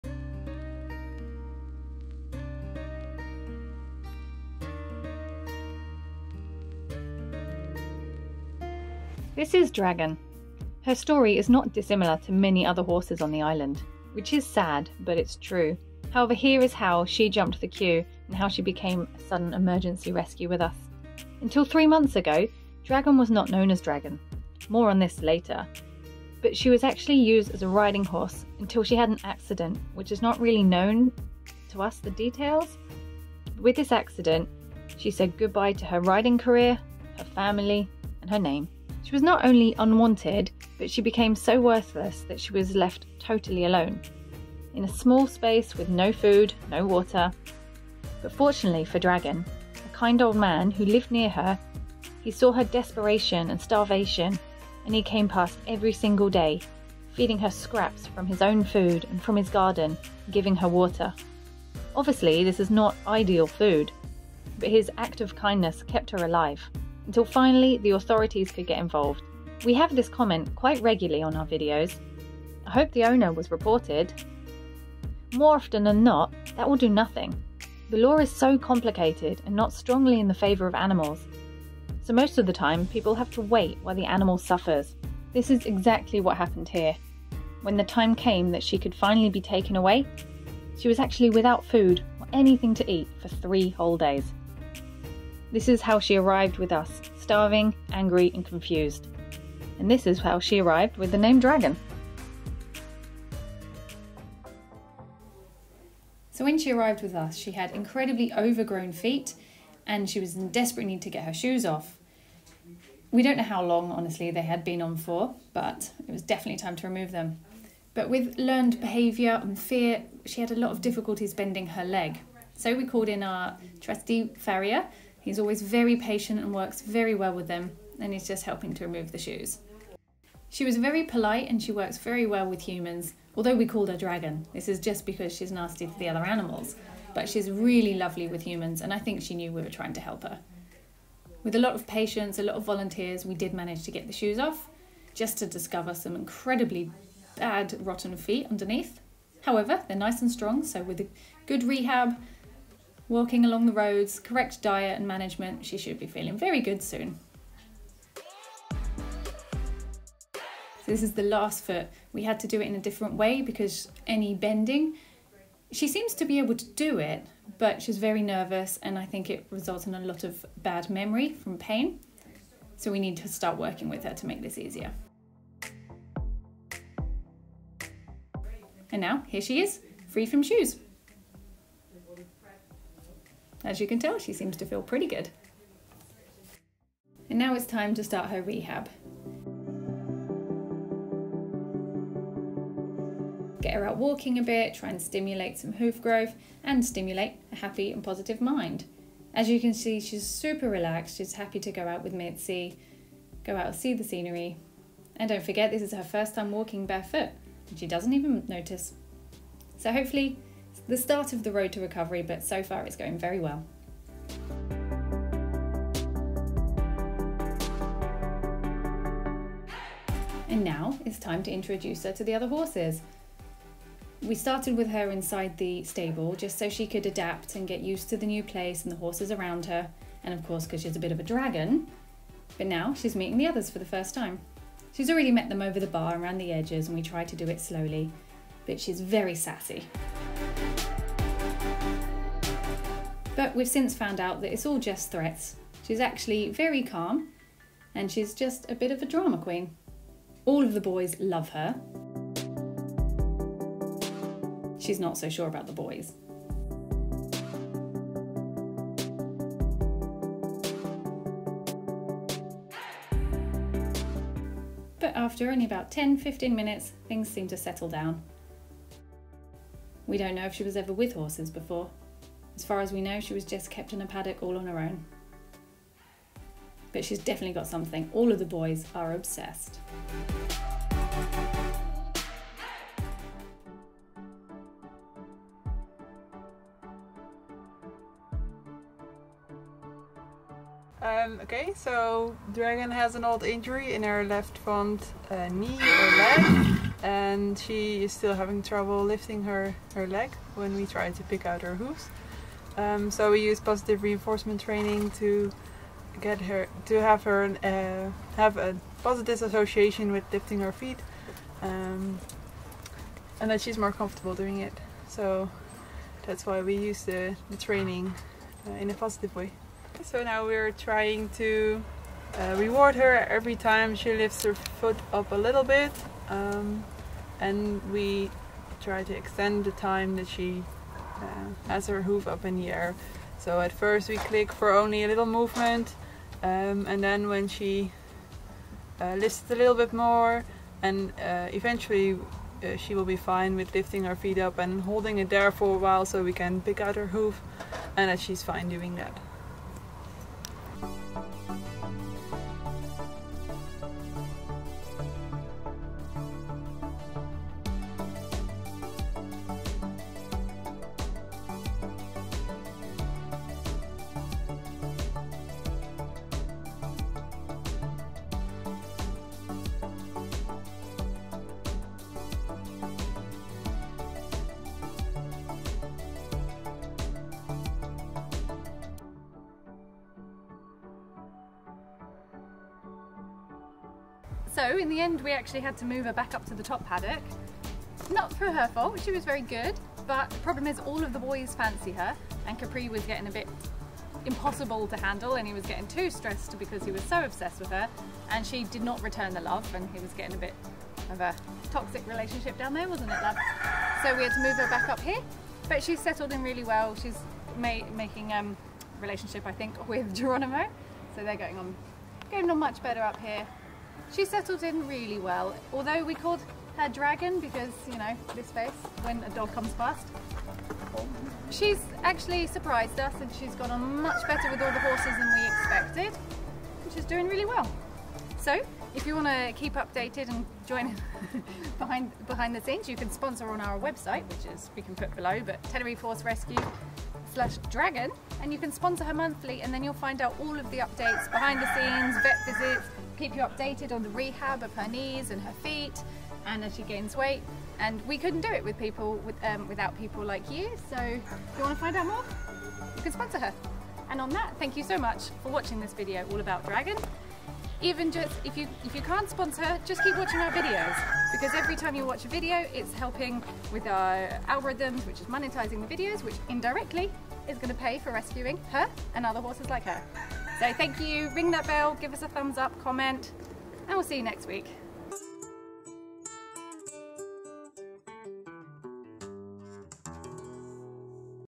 She hadn't eaten in 3 days! Meet Dragon, new horse rescue. Tenerife Horse Rescue
Why did Dragon jump the queue and become an emergency rescue?
Recently we have had so many new rescues, but unfortunately we can not take them all, even if we want to. You can help Dragon here: